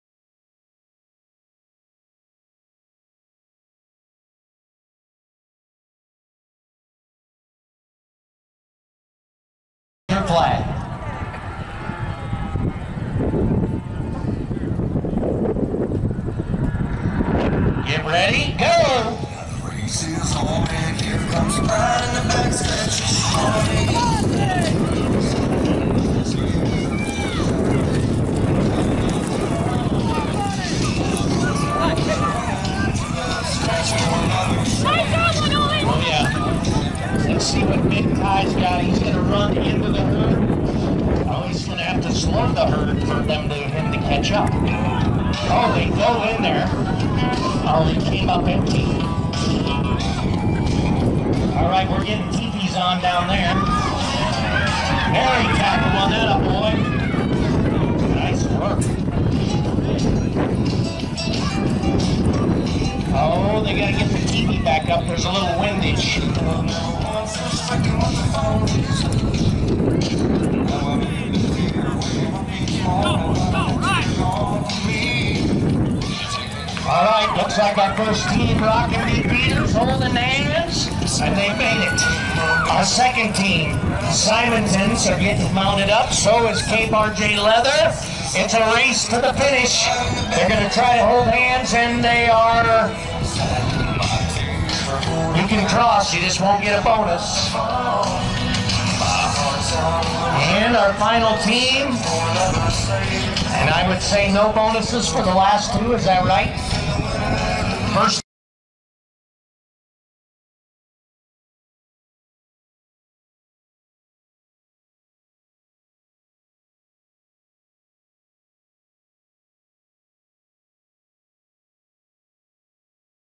Ranch Rodeo 2024 Sheep Teepeeing
Team Simanton competing in the Newell Ranch Rodeo, sheep teepeeing event. Copyright Gate to Great 2024.